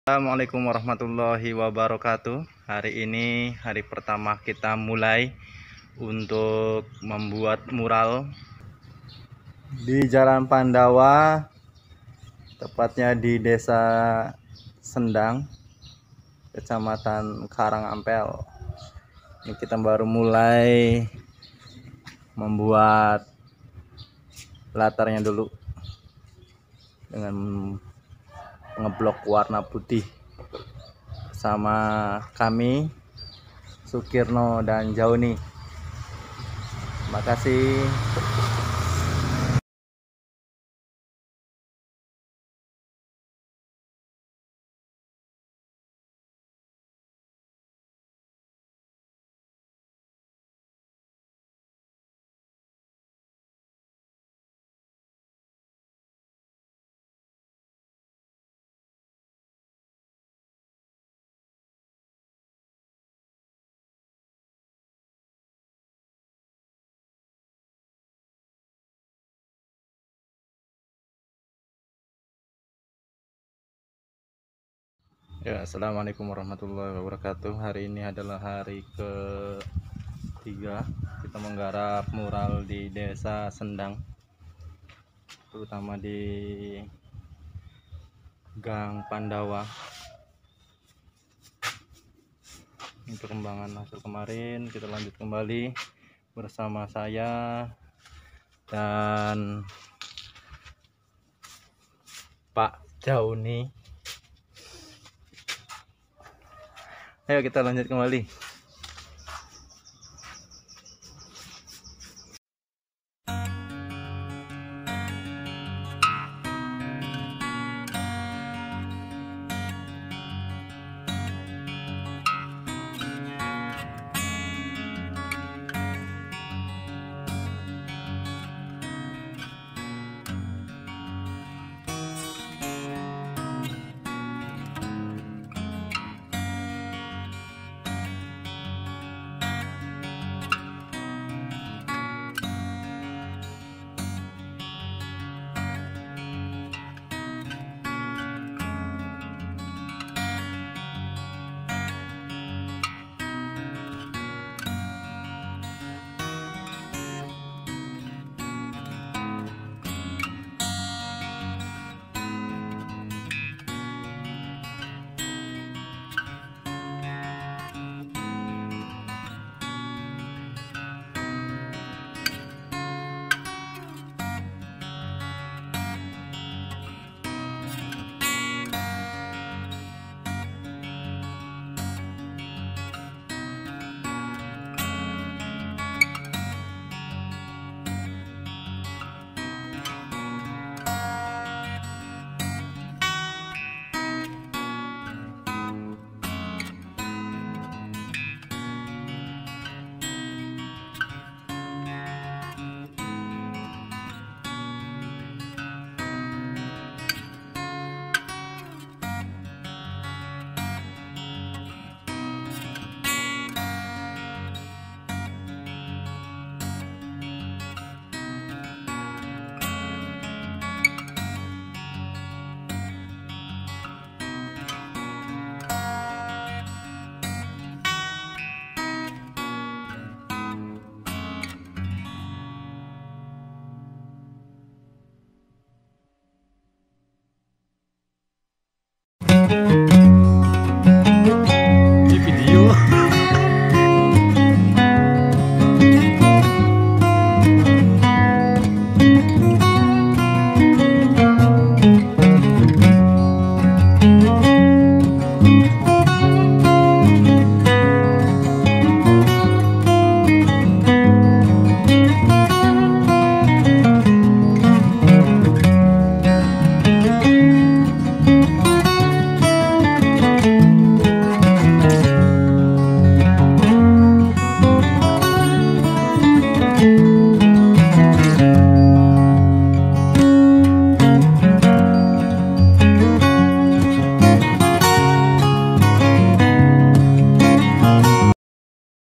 MURAL DI DESA SENDANG...
Membuat mural atau lukis tembok menyesuaikan permintaan pemesan...❤️